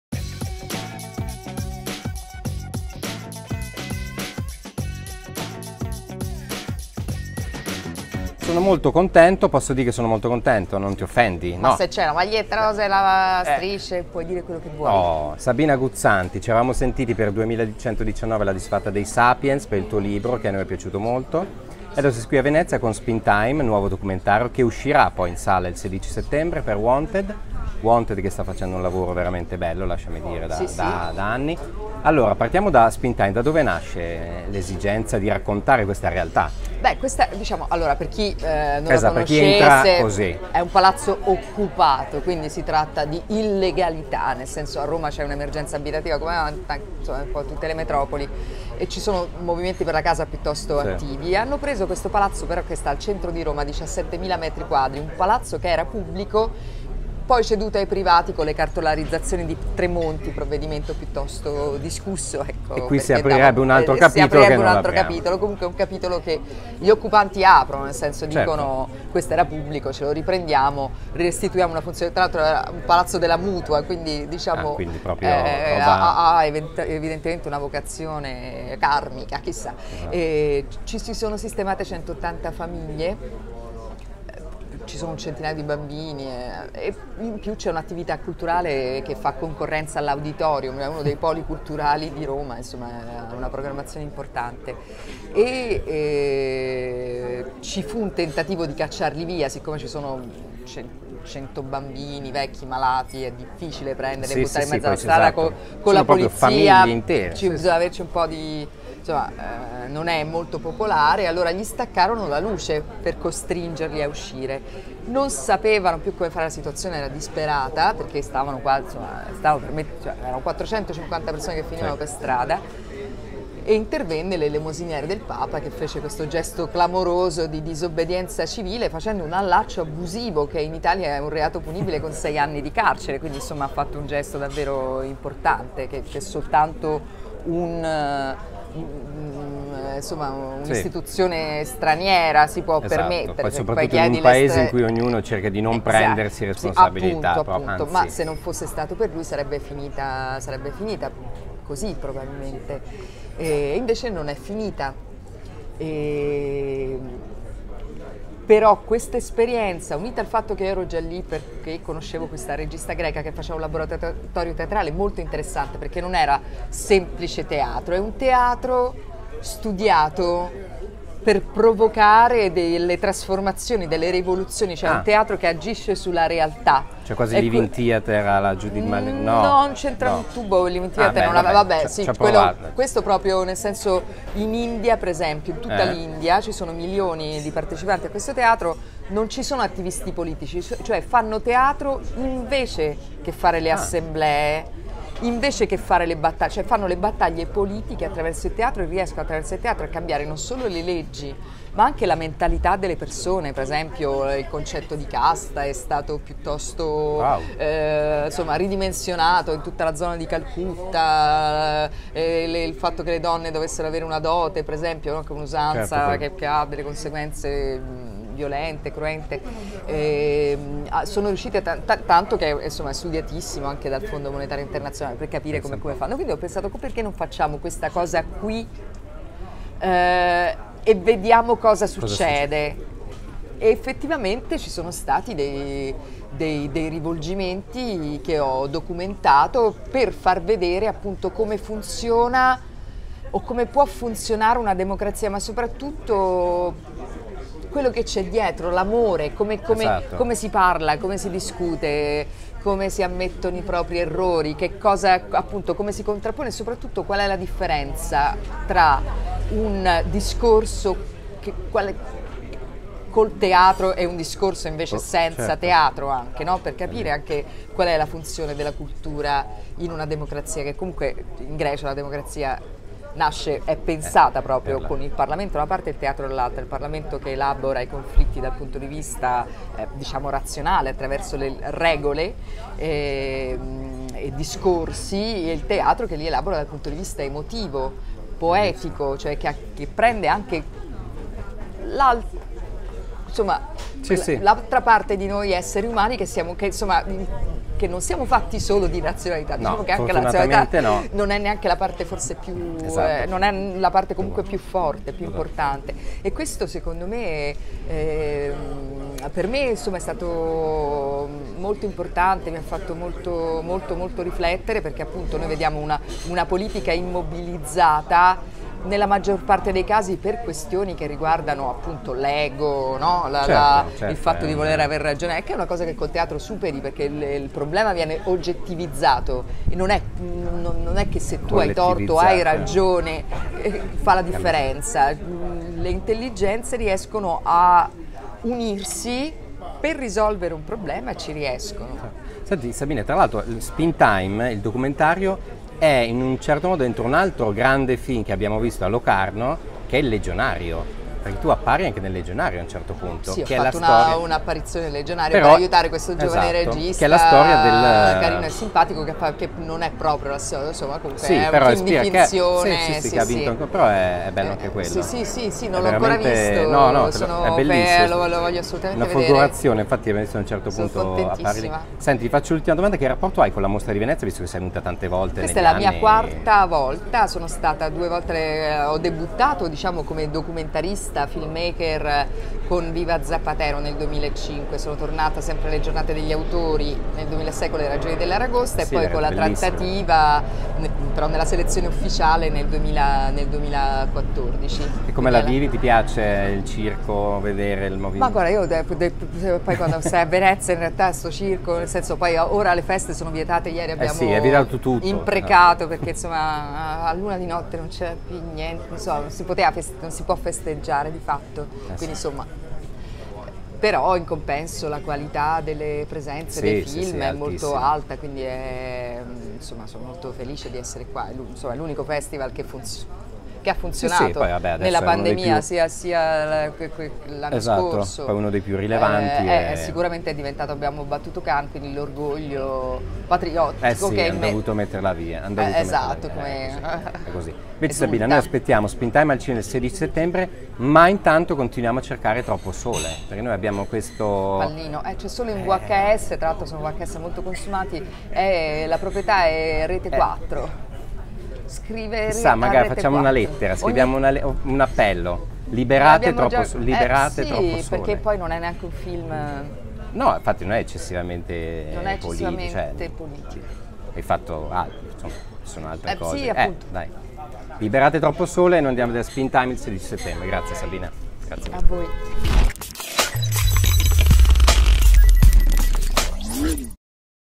SABINA GUZZANTI | Spin Time, che fatica la democrazia! | HOT CORN
Al nostro Hot Corner arriva Sabina Guzzanti a raccontare ad Andrea Morandi il suo nuovo documentario, Spin Time - Che fatica la democrazia!, in sala dal 16 settembre. Ma non solo...